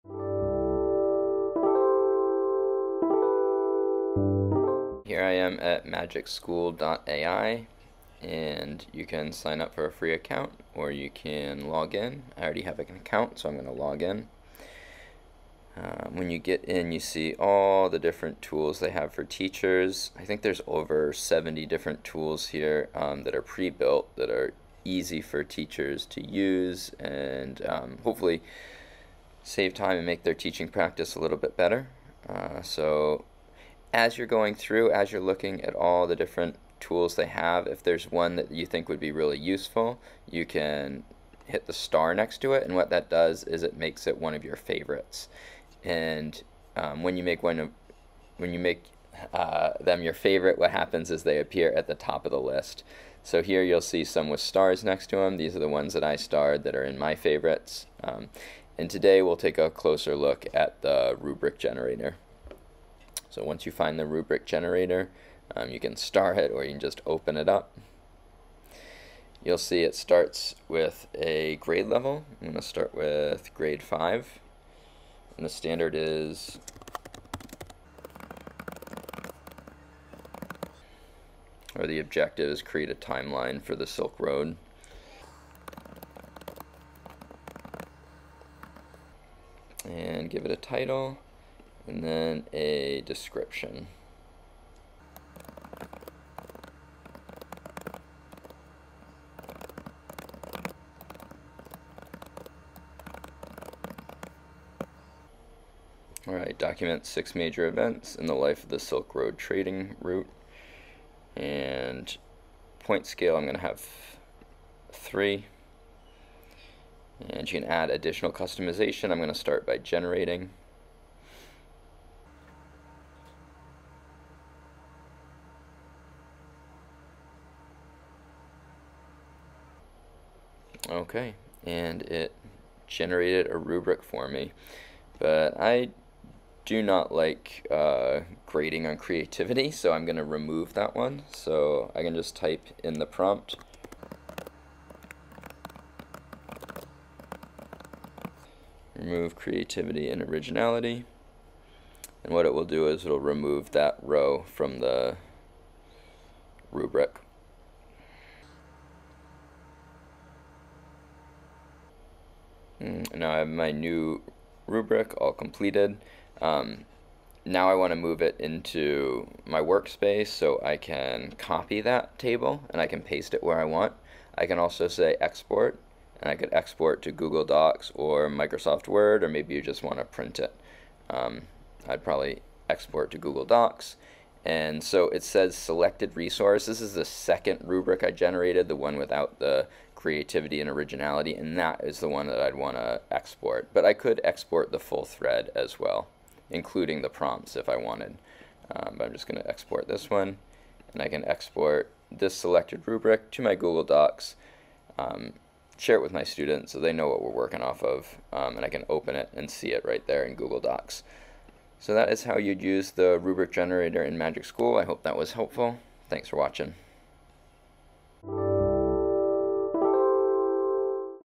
Creating a Rubric with Magic School
This is a walkthrough of how you can use AI to generate a rubric in Magic School. Magic School is a platform with AI tools for teachers.

The music is from Brake Master Cylinder